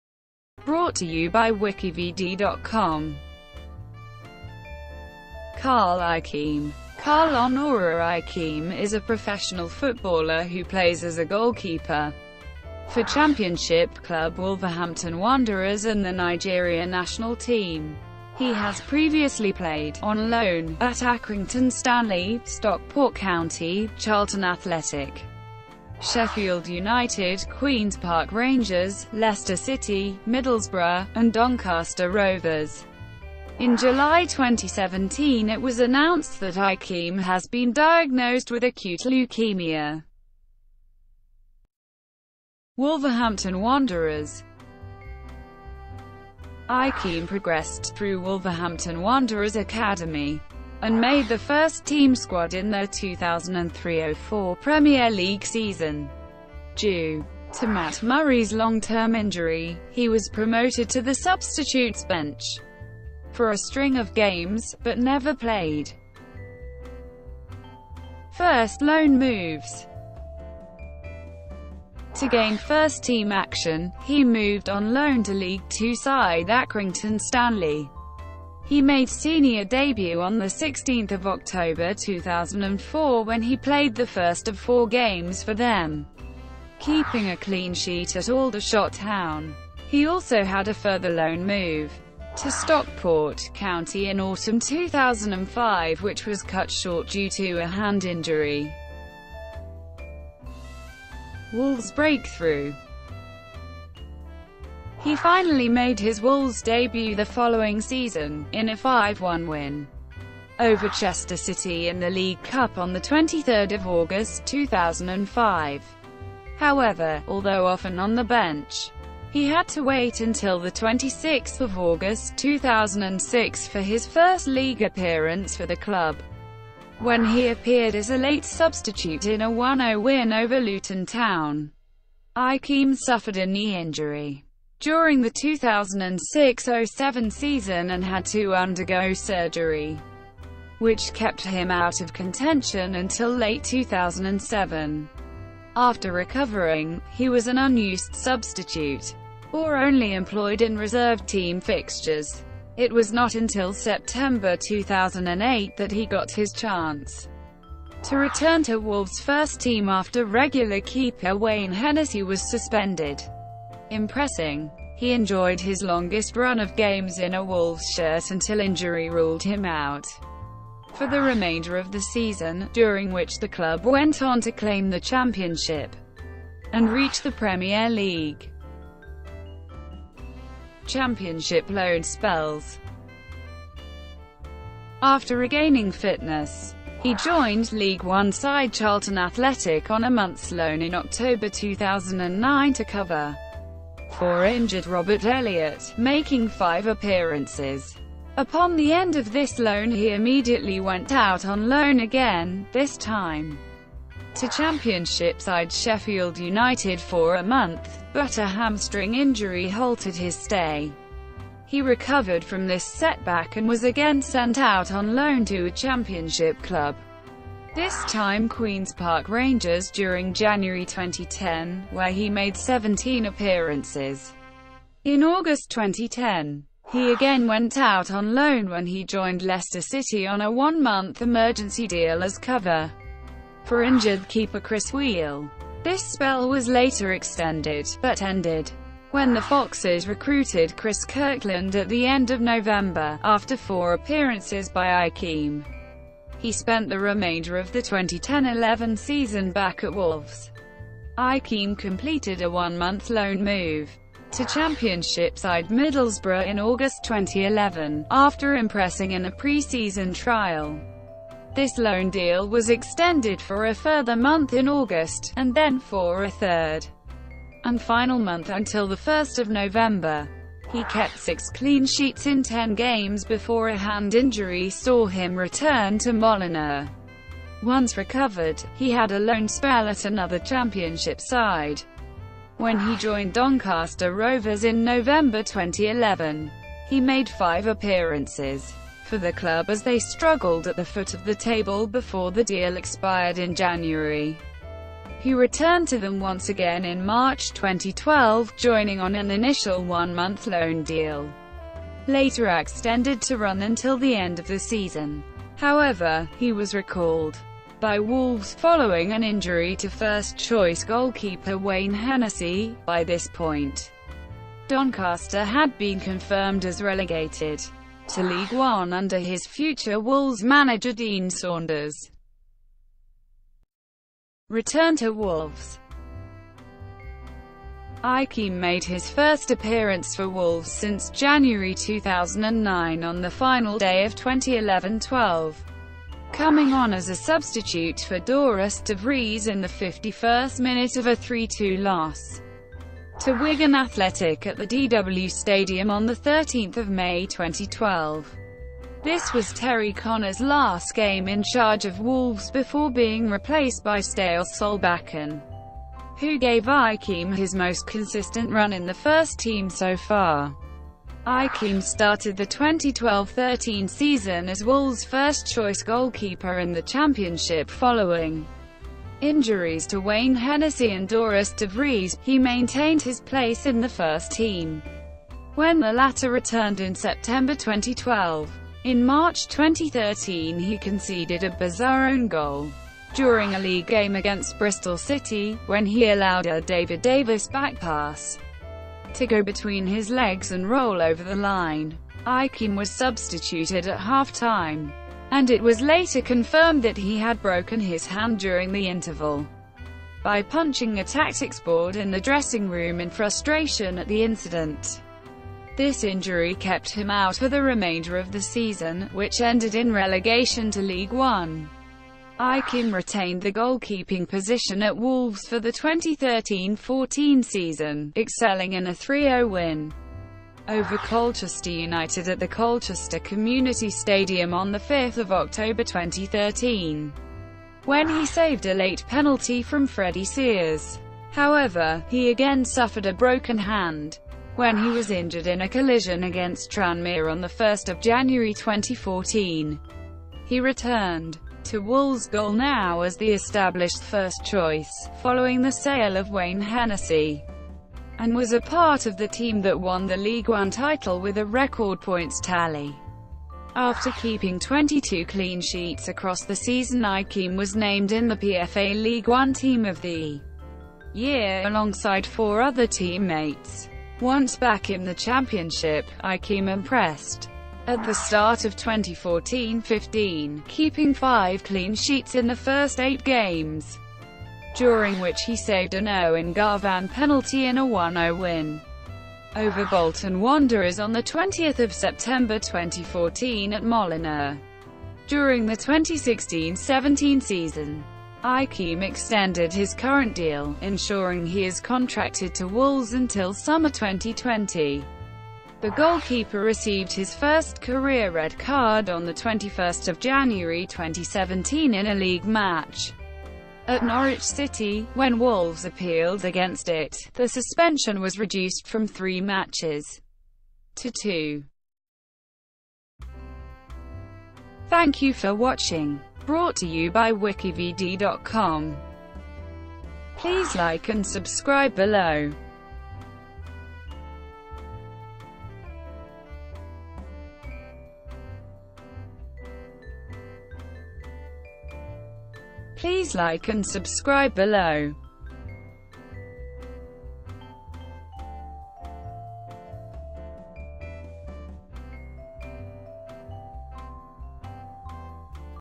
CARL IKEME - WikiVidi Documentary
Carl Onora Ikeme is a professional footballer who plays as a goalkeeper for Championship club Wolverhampton Wanderers and the Nigeria national team. He has previously played, on loan, at Accrington Stanley, Stockport County, Charlton Athletic, Sheffield United, Queens Park Rangers, Leicester City, Middlesbrough, and Doncaster Rovers. In July 2017 it was announced that Ikeme has been diagnosed with acute leukaemia.

Shortcuts to chapters:
00:00:42: Wolverhampton Wanderers
00:01:08: First loan moves
00:01:41: Wolves breakthrough
00:03:00: Championship loan spells

Copyright WikiVidi.